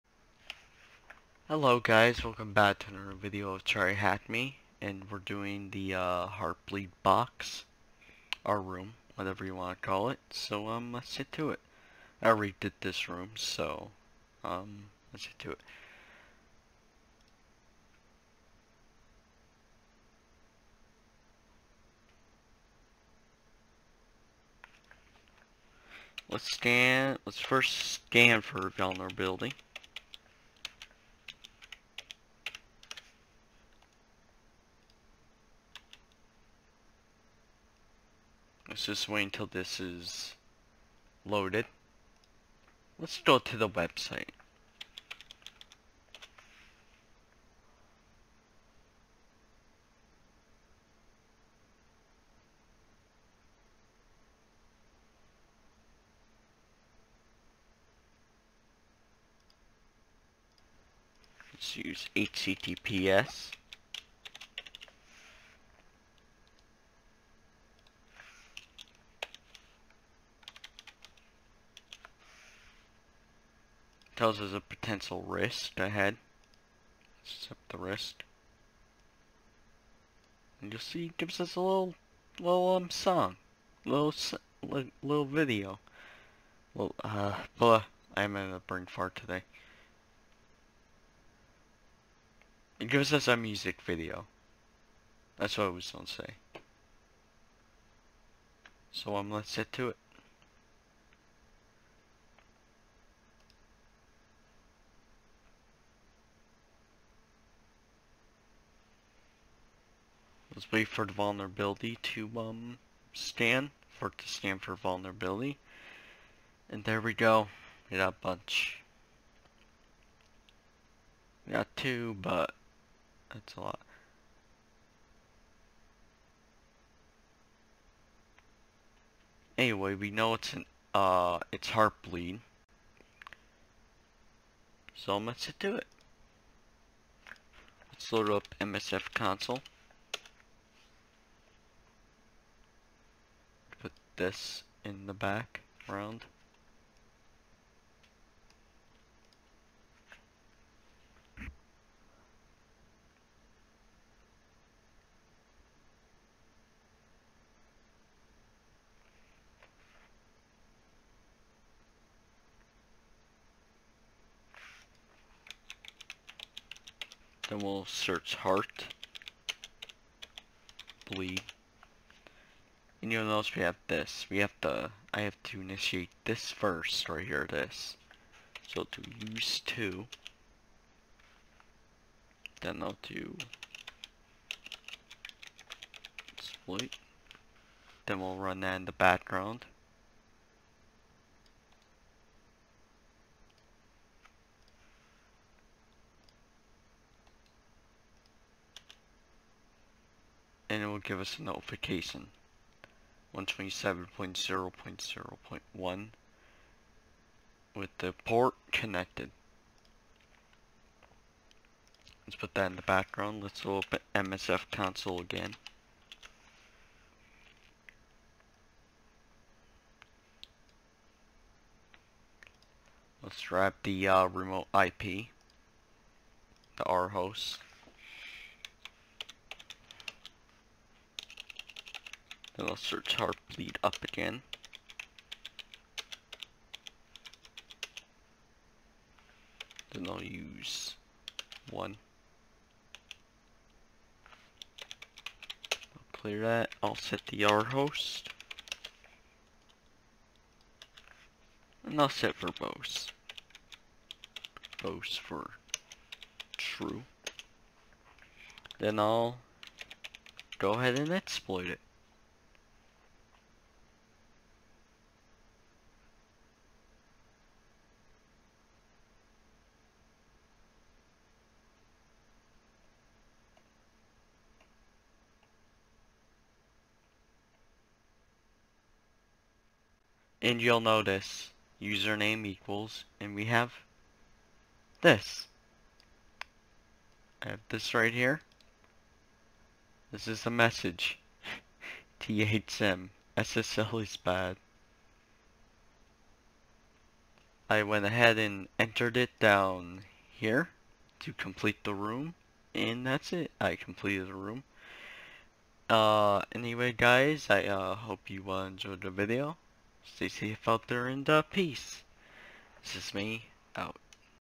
TryHackMe | HeartBleed
Hello guys welcome back to another tryhackme video where i do the HeartBleed room in tryhackme anyway i hope you enjoy the video

Also consider joining the auth gaming discord server for video updates and chat there too